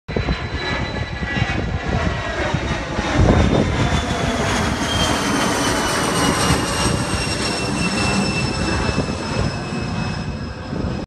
Off Topic - Wicked Orange Sunrise at TPA
A strange morning sky at sunrise on October 23, 2017 at Tampa International Airport.  It looked much more like the colors of a sunset than a sunrise. This is looking east across runway 1R/19L from near the Economy Parking garage.